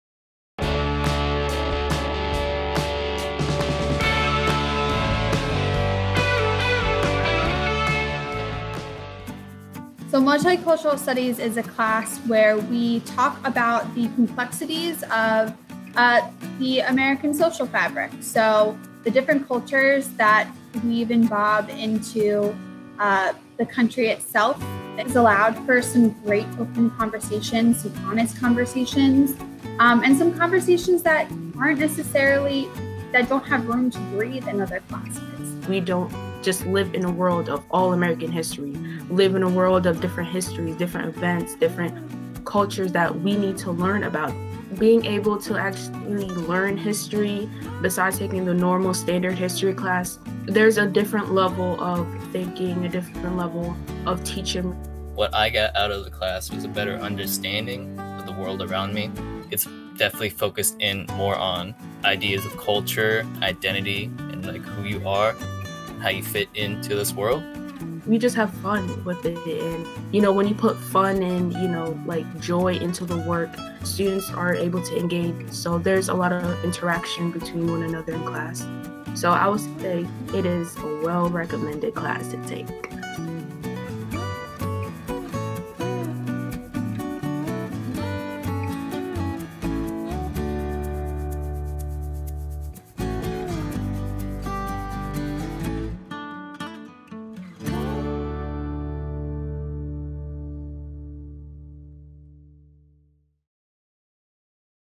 HTSD CURRICULUM SPOTLIGHT: Multicultural Studies
The HTSD Curriculum Department is highlighting some high school classes that you might not know about. Check out this video spotlighting the social studies class, Multicultural Studies.

Hamilton Township School District
Mercer County, New Jersey